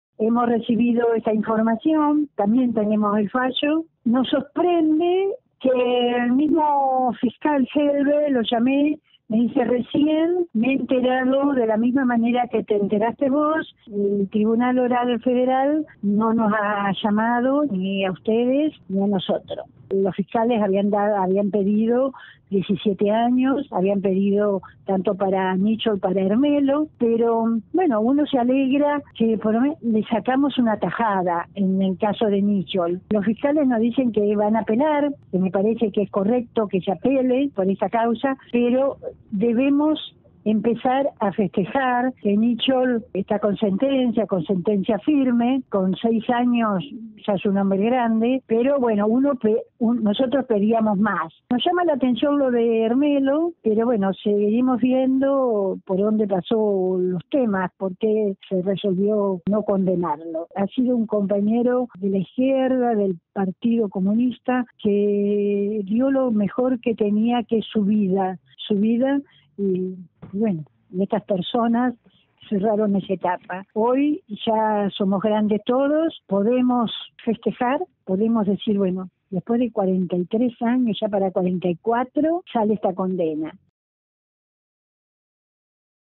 Fallo histórico
Después de 44 años, el Tribunal Federal en lo Criminal del Chubut condenó a 6 años de prisión al ex comisario Tito Nichols y absolvió al exmarino Hernán Álvaro Hermelo por la desaparición forzada del docente Elvio Ángel Bel en 1976.